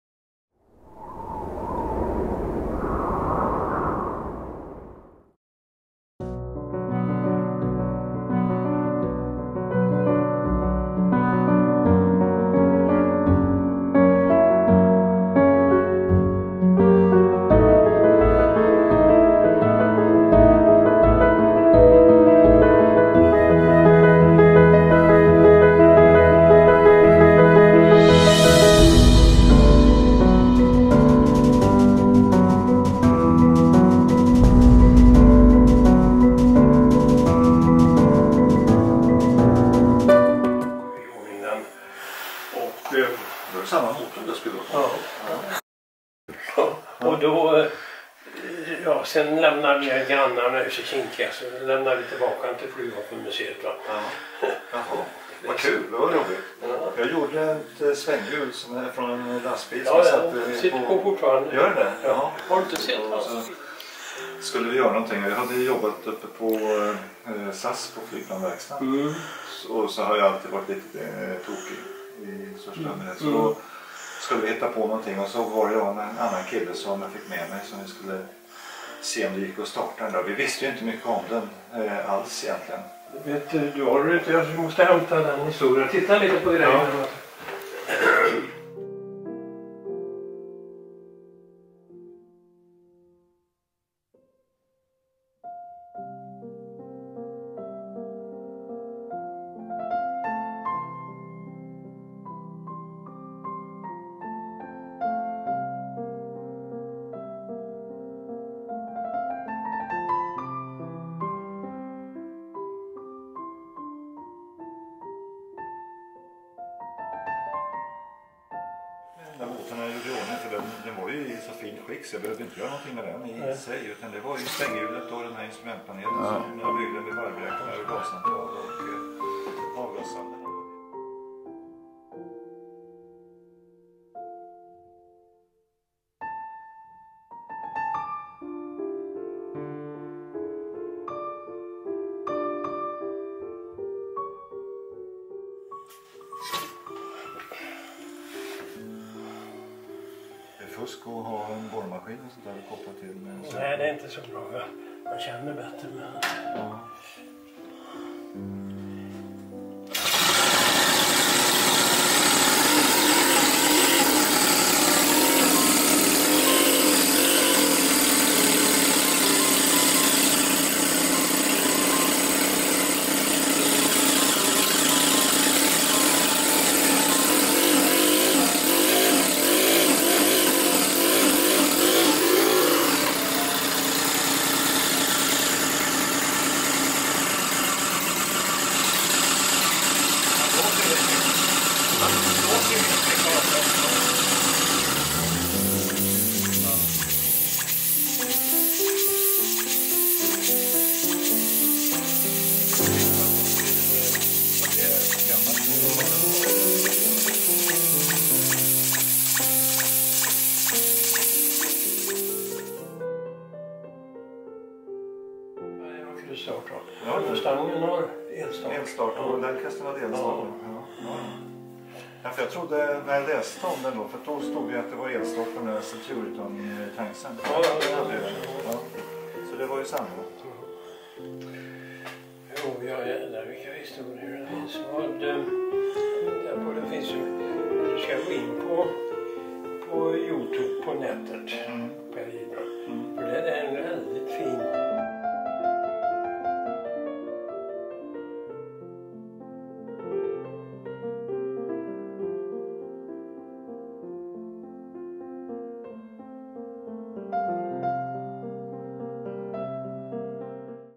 Motorgeni möter Flygaress
Motorgeniet Per Gillbrand "Saab Turbo" får besök av Flygaress Ulf Cedermark "Gottröra 1990"